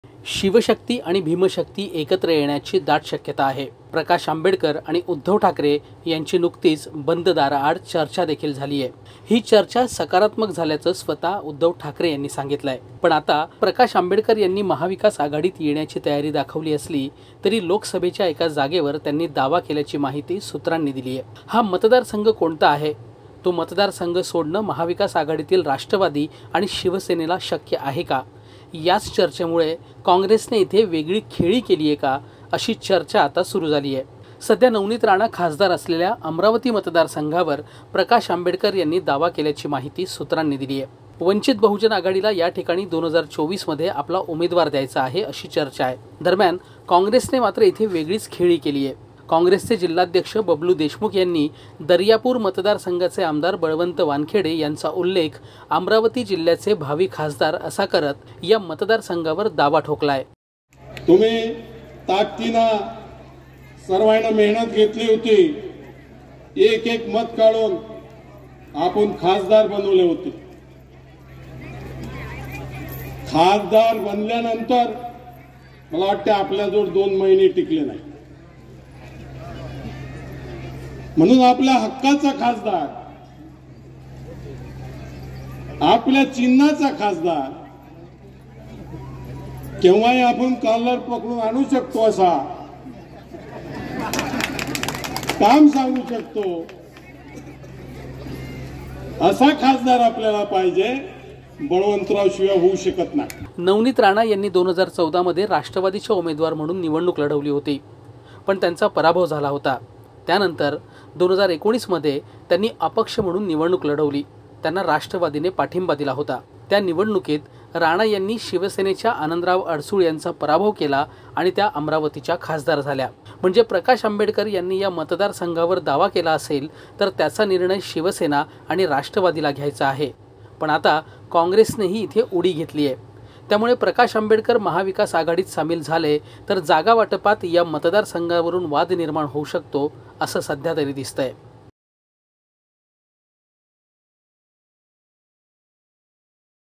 MVA VBA Alliance : एका मतदारसंघावरुन प्रकाश आंबेडकर आणि महाविकास आघाडीची चर्चा रखडली?
शिवशक्ती आणि भीमशक्ती एकत्र येण्याची दाट शक्यता आहे. प्रकाश आंबेडकर आणि उद्धव ठाकरे यांनी बंद दाराराआड केलेली चर्चा सकारात्मक असल्याचे स्वत उद्धव ठाकरे यांनी सांगितले आहे. पण आता प्रकाश आंबेडकर यांनी महाविकास आघाडीत येण्याची तयारी दाखवली असली तरी लोकसभेच्या एका जागेवर त्यांनी दावा केल्याची माहिती सुत्रांनी दिलीये...
(Sachin vo)